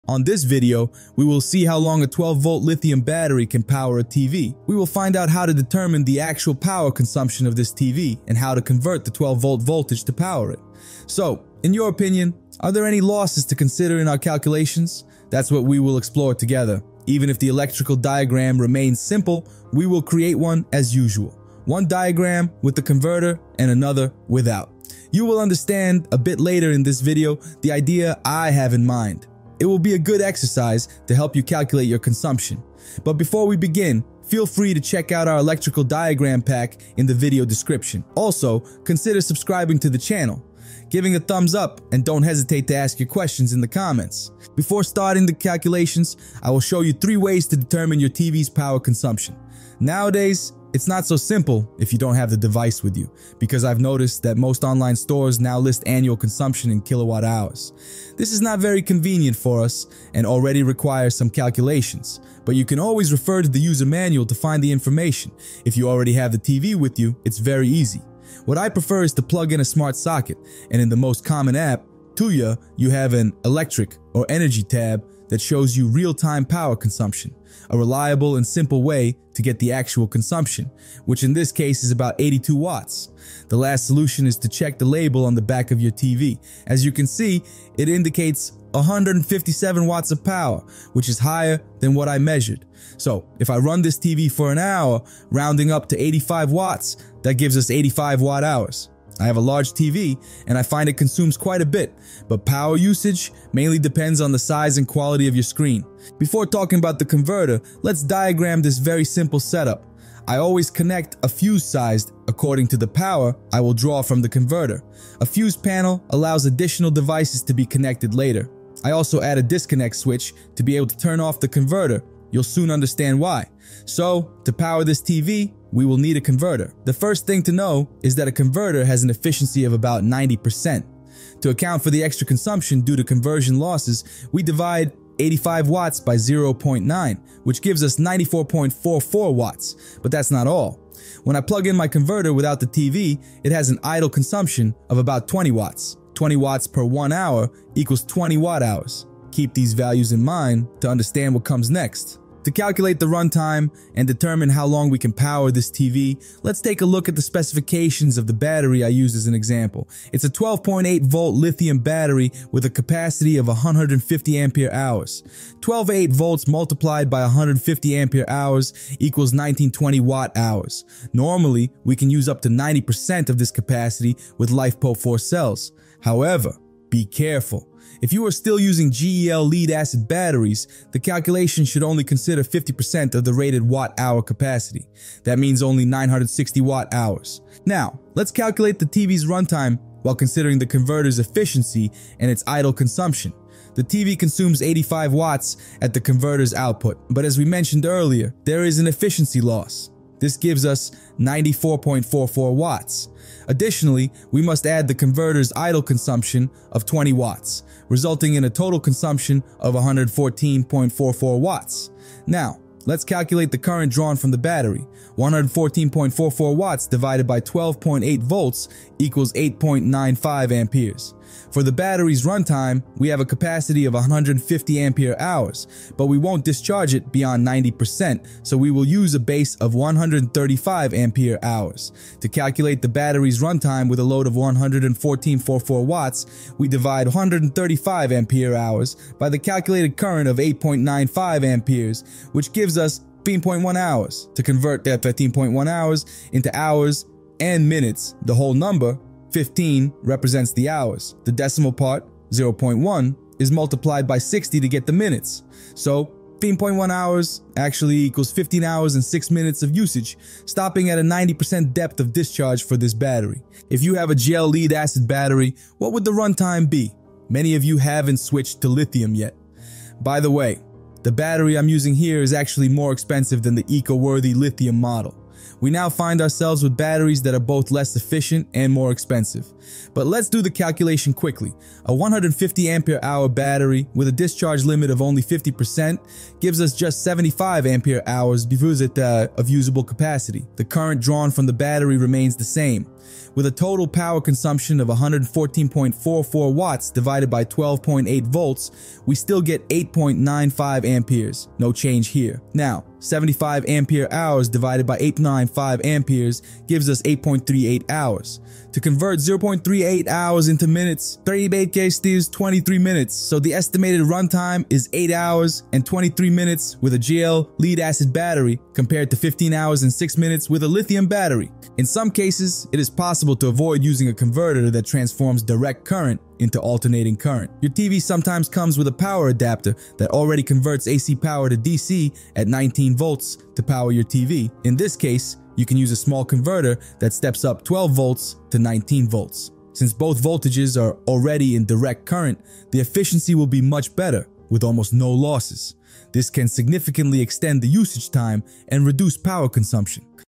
How Long Can a 12V 150Ah Battery Run a TV? - Lithium VS Gel Lead Acid
How Long Can a 12V Battery Power a TV?
Get your Electrical Diagram Pack!
Description :
Are you wondering how long a 12V 150Ah Lithium LiFePO4 battery can power your television? In this video, we do the precise calculation by considering the actual power consumption of the TV, the efficiency of the inverter, and potential losses.
How to measure the actual power consumption of your TV with practical tips, whether it's indicated on the label, the manual, or via a smart plug.
The detailed wiring diagram with or without an inverter, including essential components like the fuse and disconnect switch.
The calculation of autonomy, taking into account the efficiency of the inverter and the idle consumption.
Comparison between a Lithium battery and a GEL battery: discover the difference in autonomy and why the choice of battery type is crucial for your off-grid electrical system.
Tips for optimizing autonomy by reducing conversion losses to alternating current.

▬▬▬▬▬▬▬▬▬▬▬ **VIDEO CHAPTERS** ▬▬▬▬▬▬▬▬▬▬▬
00:00 Introduction
00:39 Finding the actual consumption
01:41 Wiring diagram
02:03 Inverter efficiency
02:28 Battery capacity
03:01 Consumption calculation
04:16 What about a GEL battery?